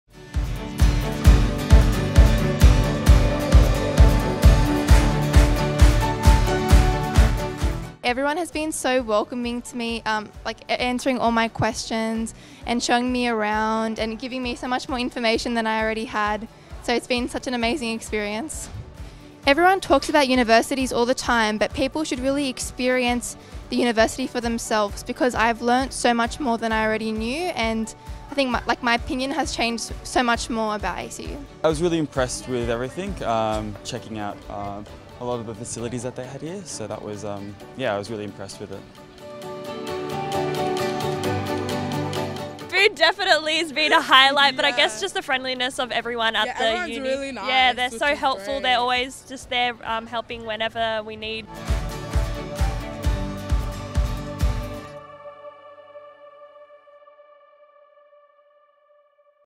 ACU I North Sydney Open Day I 2017 Highlights
We chatted to visitors from ACU's Open Day on North Sydney campus to find out why they chose to attend.